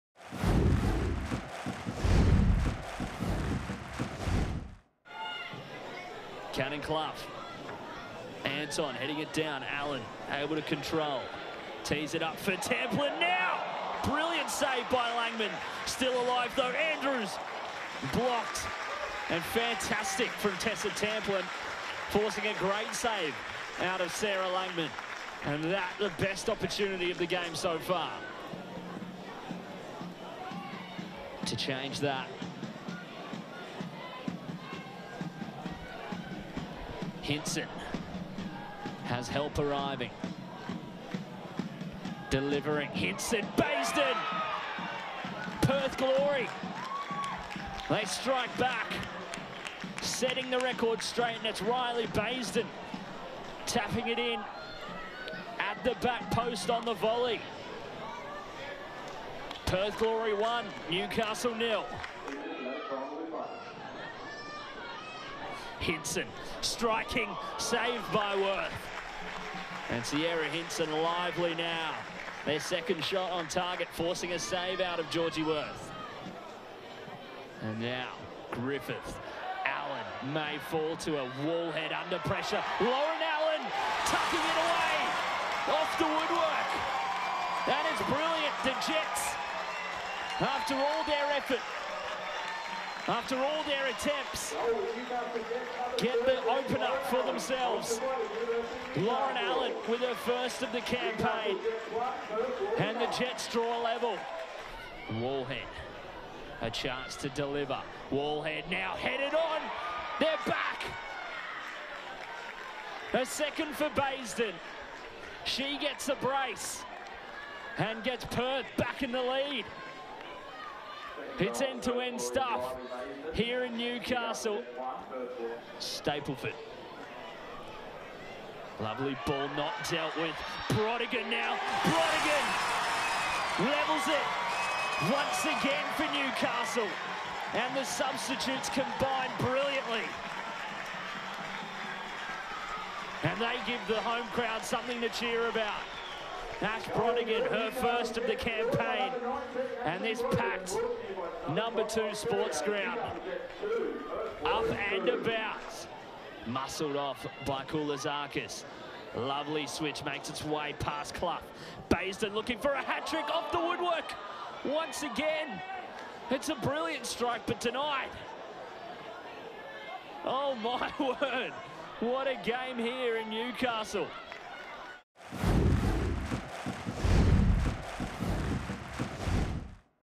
Jets v Glory ALW Round 3
Highlights from the Jets Round 3 highlights against Perth Glory at No. 2 Sportsground in Newcastle.